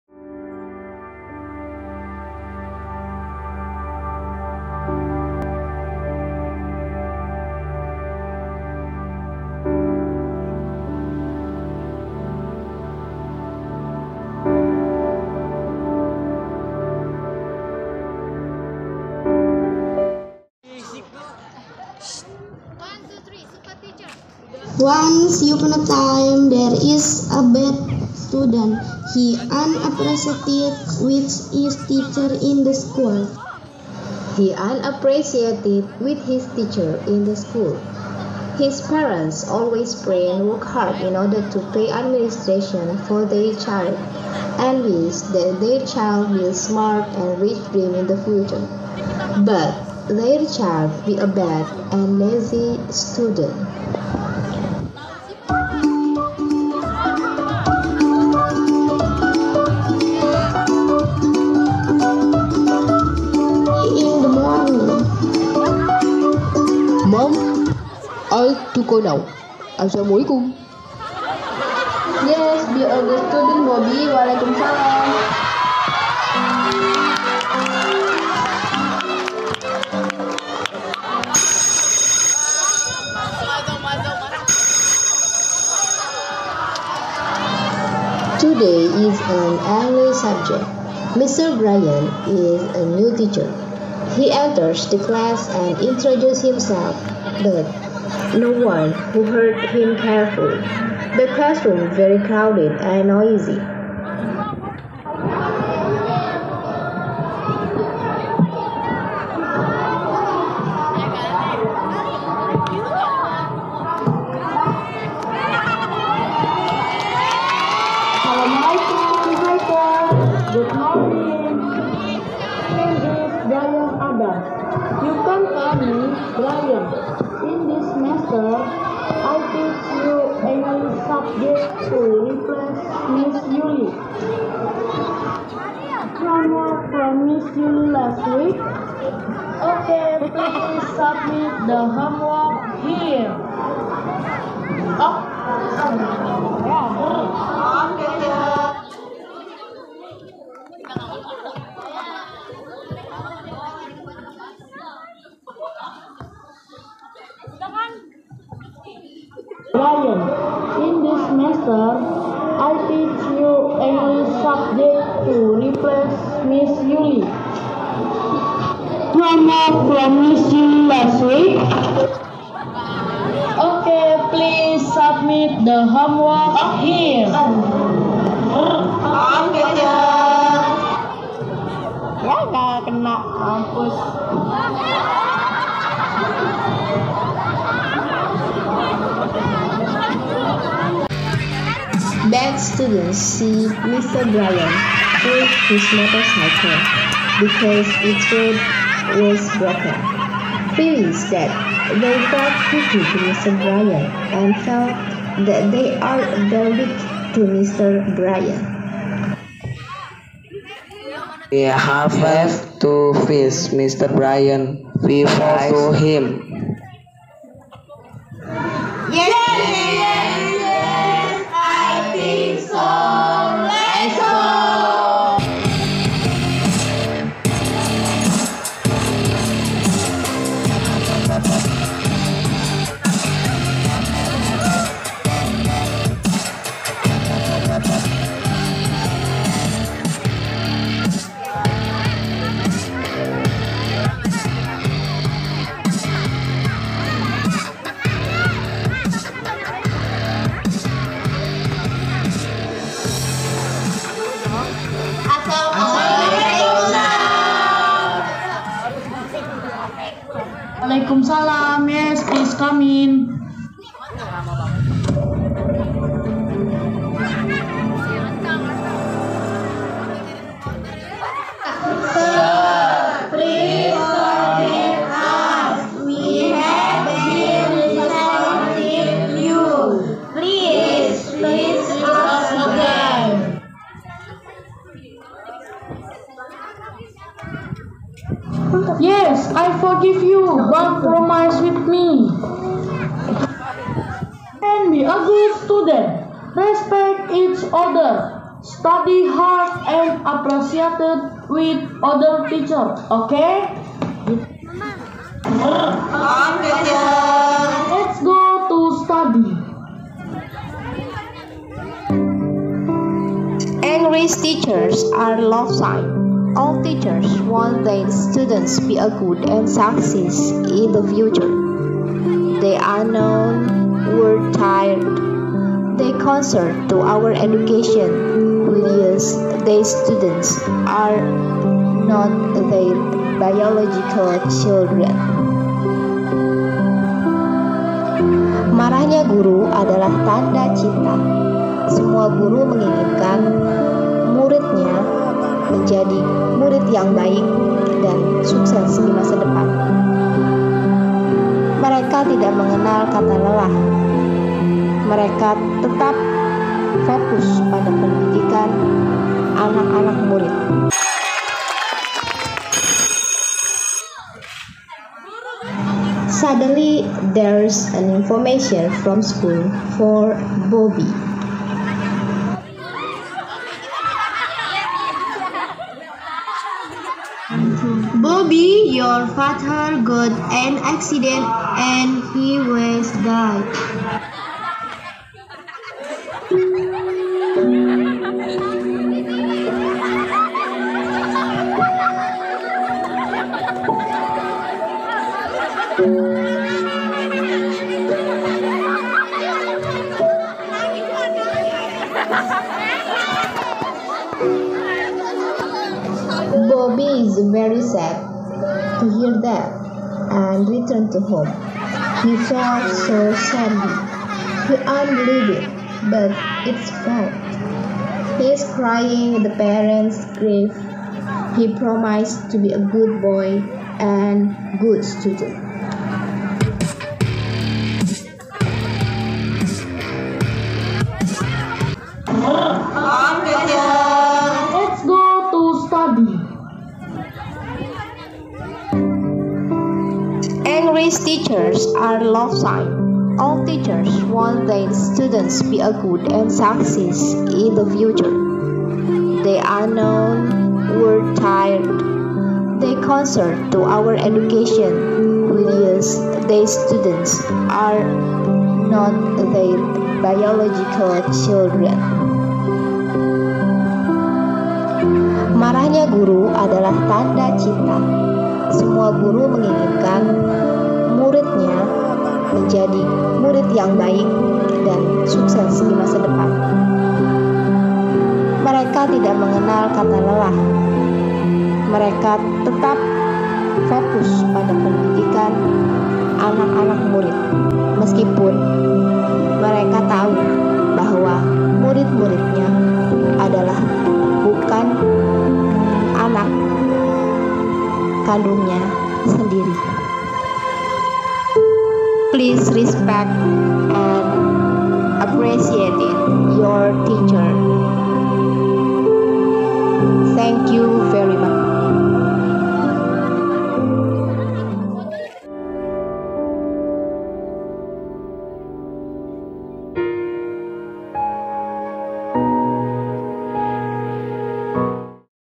Drama Musikal Bahasa Inggris hari guru / MTS Khairul Falah Bekasi
Drama ini menceritakan tentang sekumpulan anak nakal yang tidak menghormati orang tua maupun guru dan adanya pesan Allah yang ia hadapi maka mereka berubah menjadi anak yang baik dan patuh.